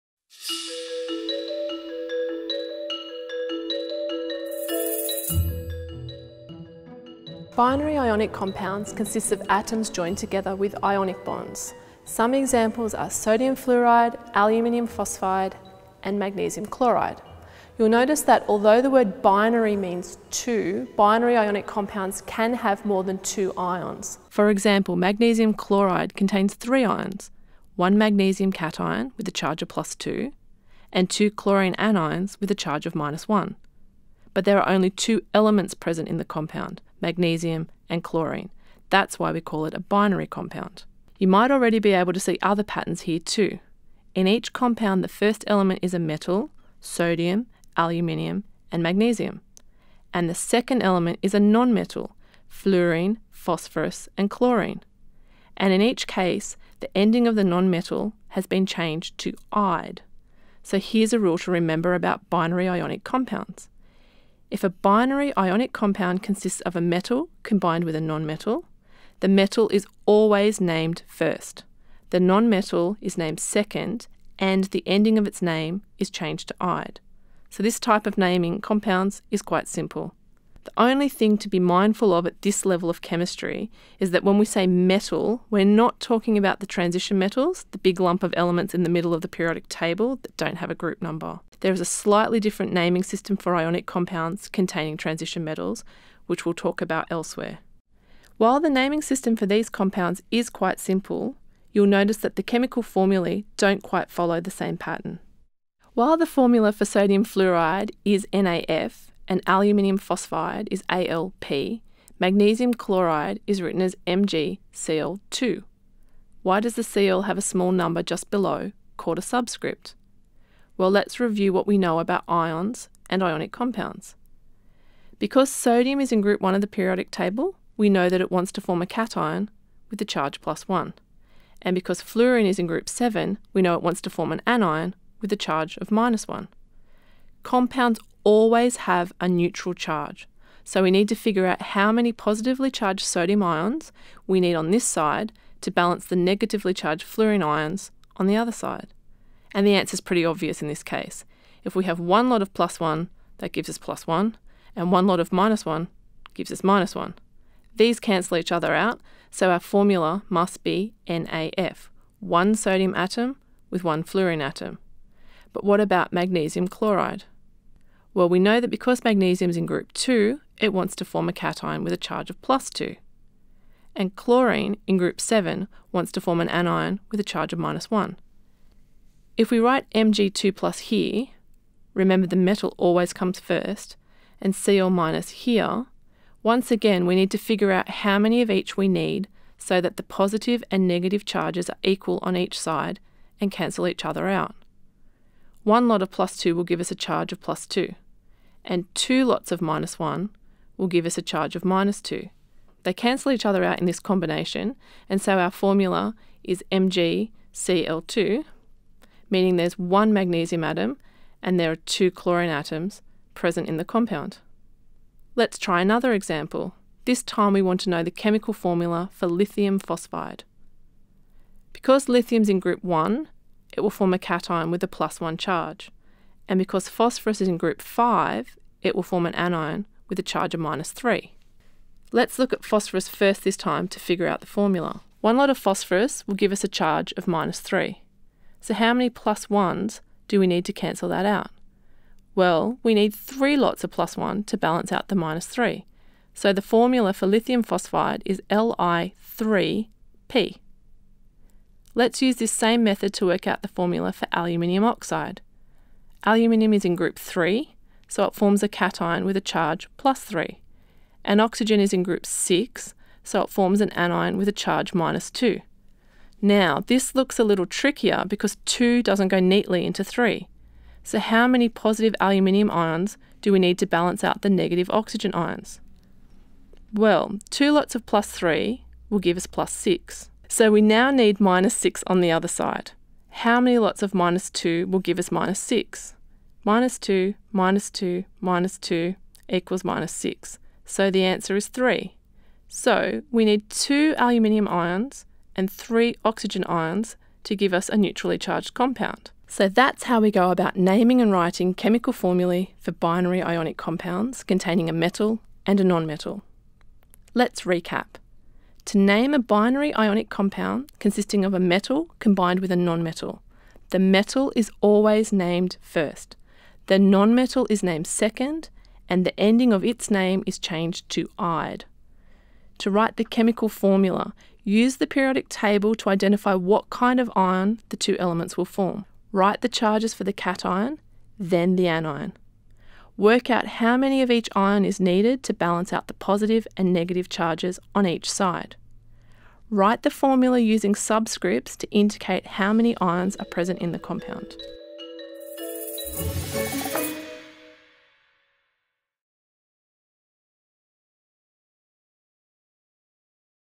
Module 6: Naming ionic compounds
Watch as Michelle introduces you to how to name ionic compounds.